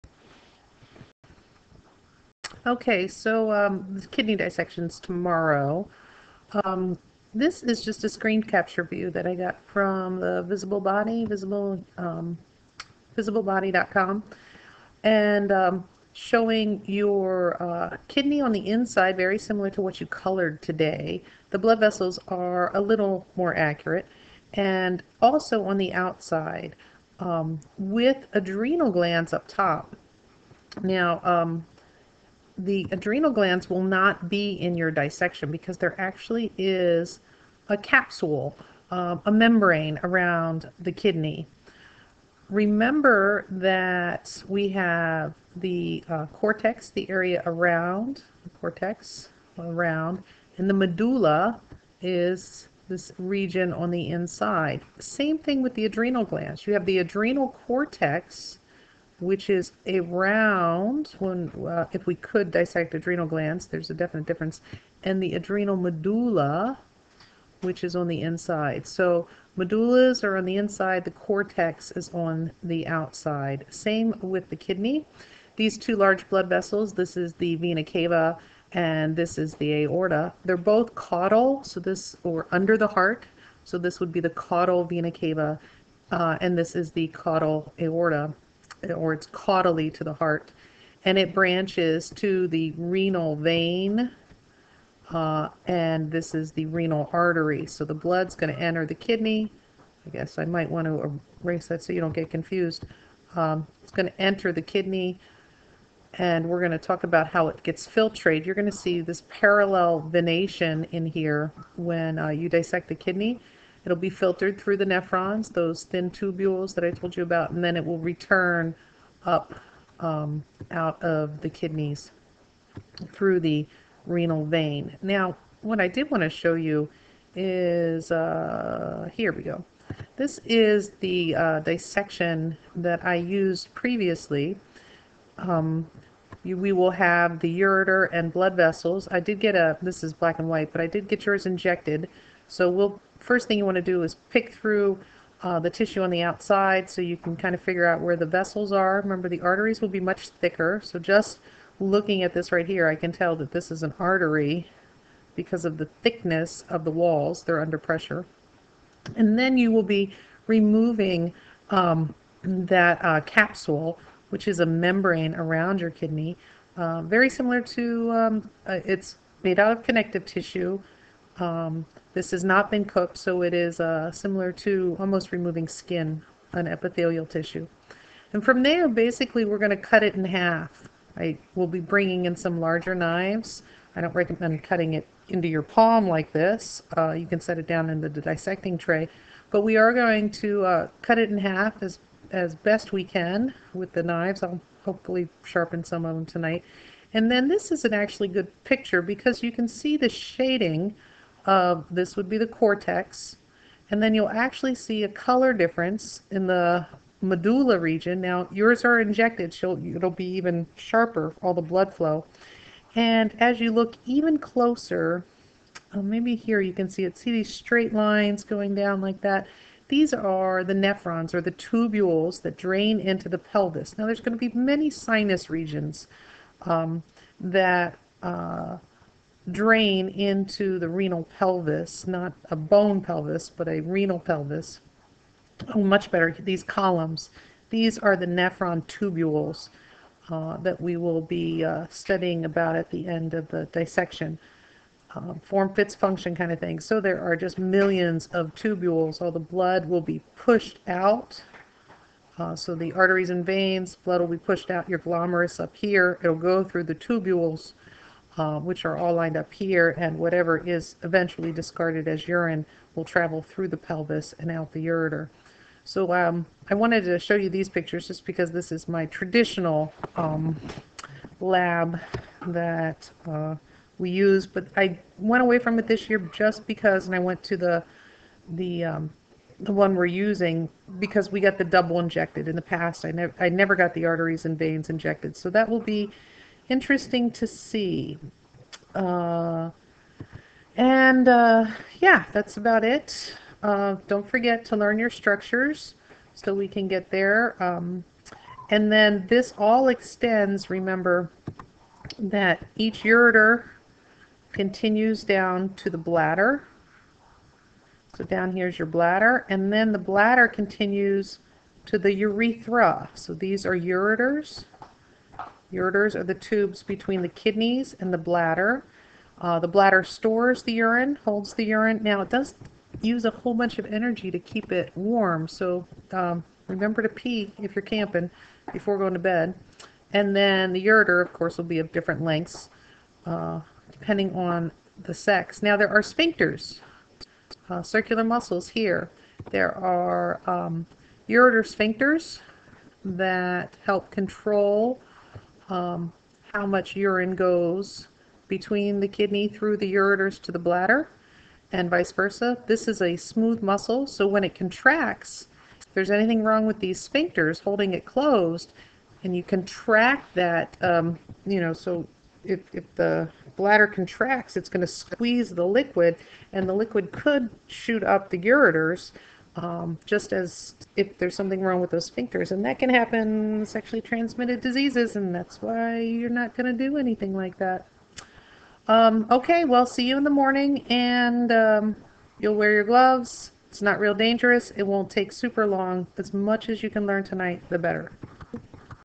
PreKidneyDissection.wmv
Quick explanation of some kidney structure.  Review the structures before lab.